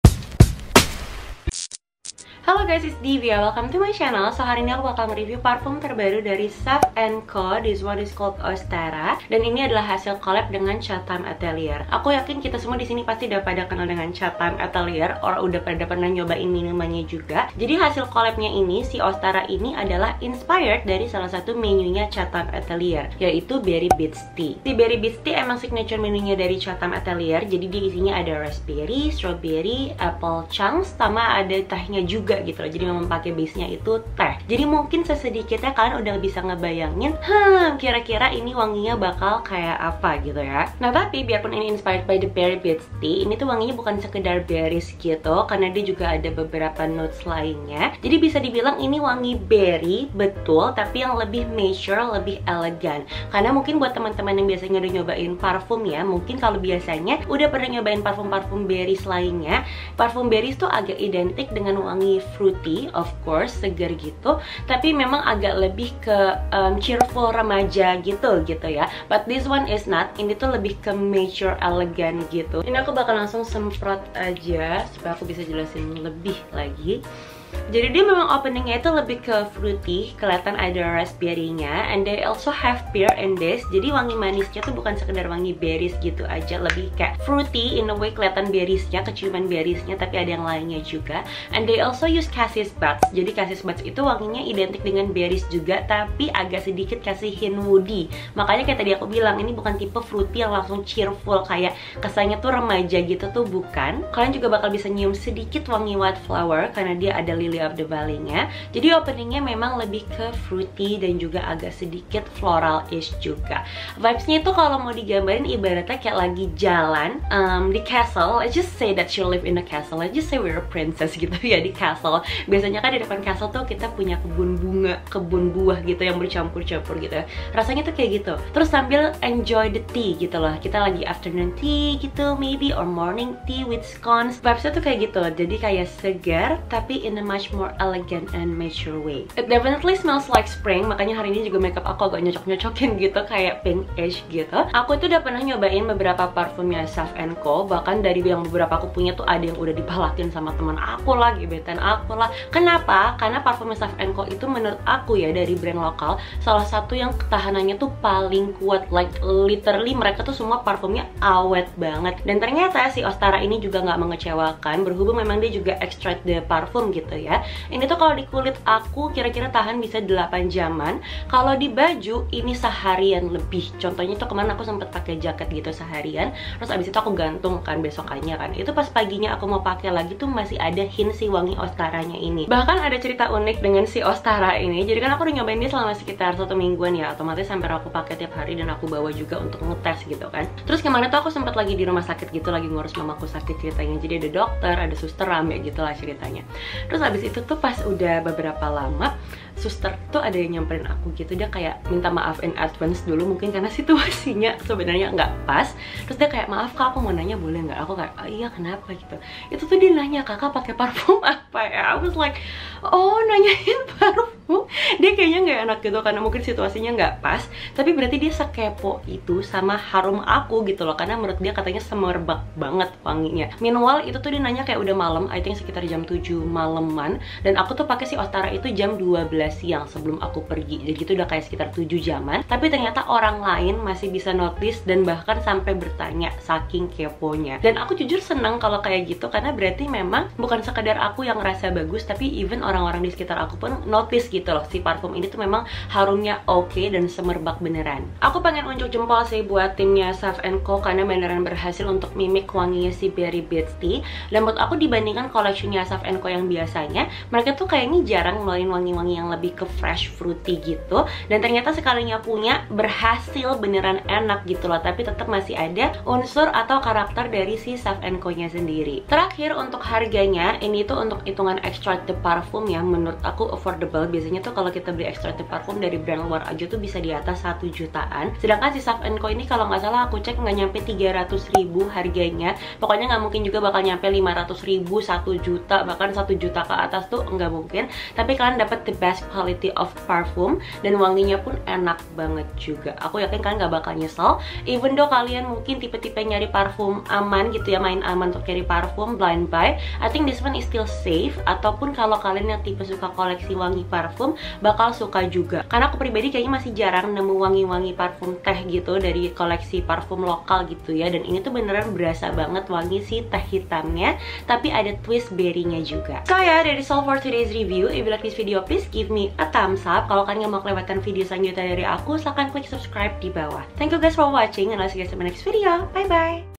Review Saff & Co x Chatime Atealier Ostara Extrait De Parfum | Parfum lokal wangi teh?
Hi guys, it’s divia!

Product i used: Saff & Co. x Chatime Atealier Ostara Extrait de Parfum Rp 199.000,00